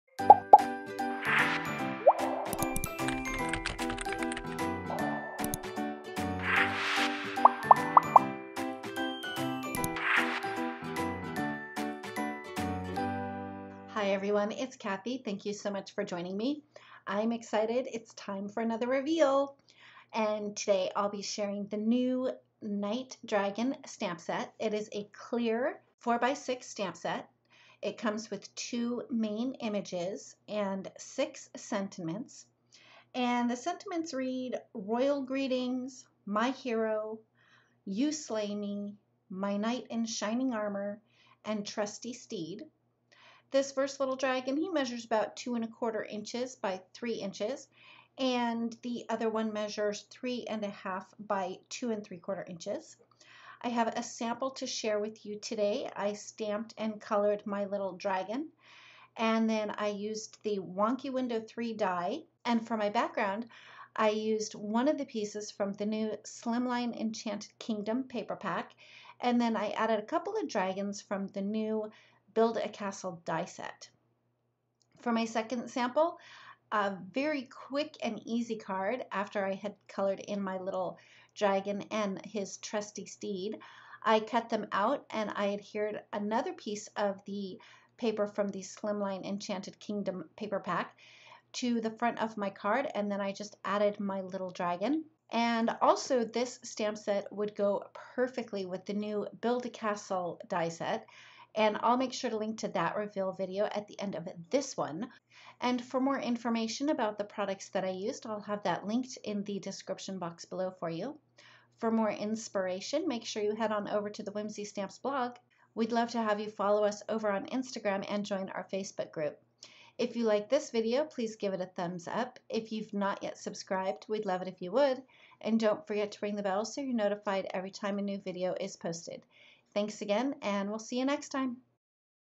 WHIMSY STAMPS PRODUCT REVEAL: Knight Dragons Stamp Set
Oh, hi! I'm so excited to share the new Knight Dragons Stamp Set with you today!
CLICK SHOW MORE for product links! 👇🏻👇🏻👇🏻